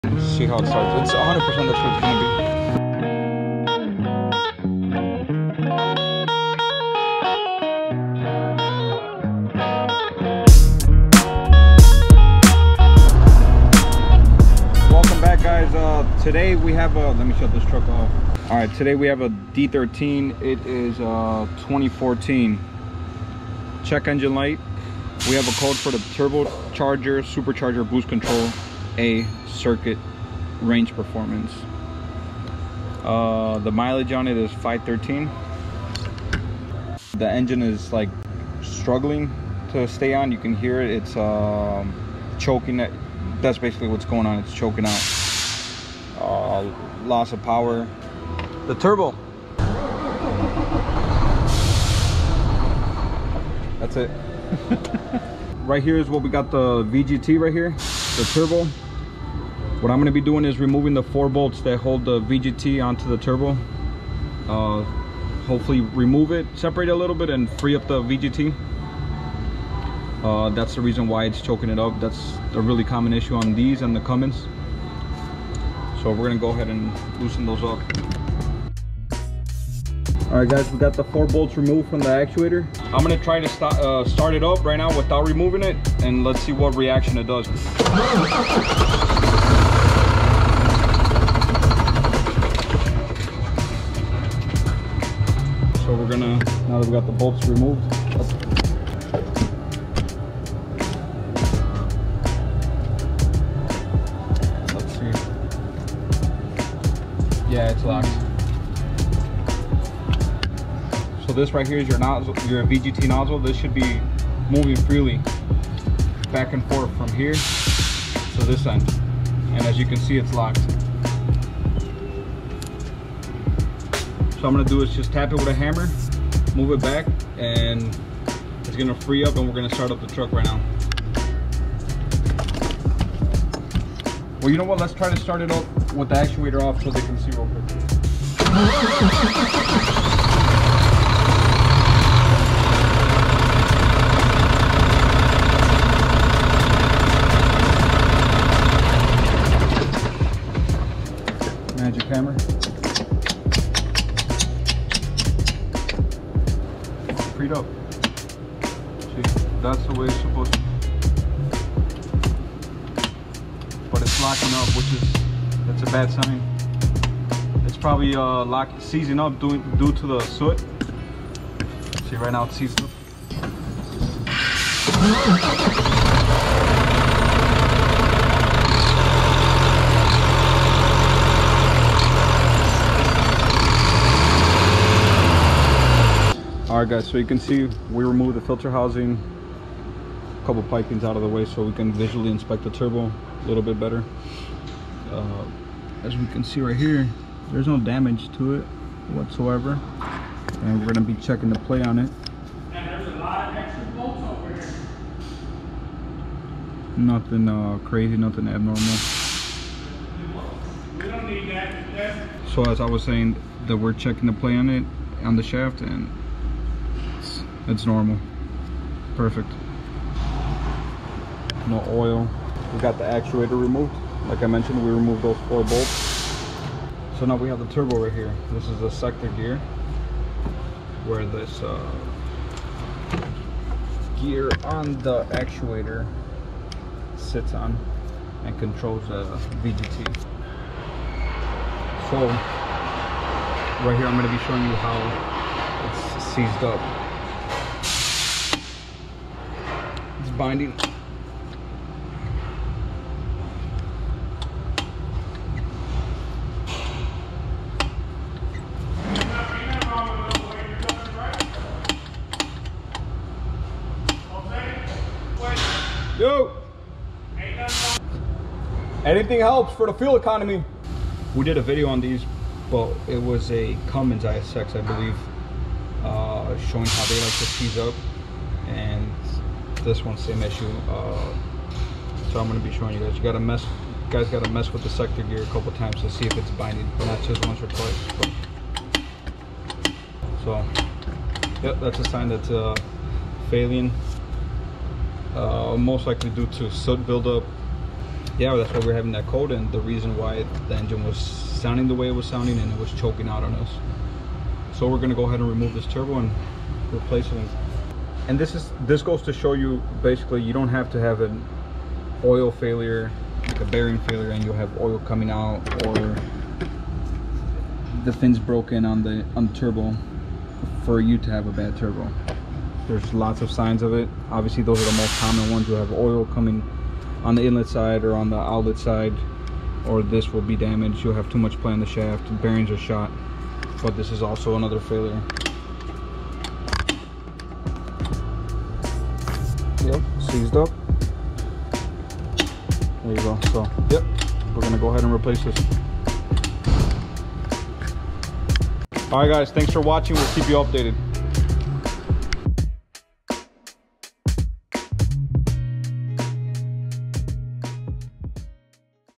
Semi truck keeps shutting down. Check This.
Hey truckers! This truck was choking after it was turned on. Tune in to see how Jose troubleshoots it.

Interested in joining our team? We are Hiring Drivers, Dispatchers and Mechanics!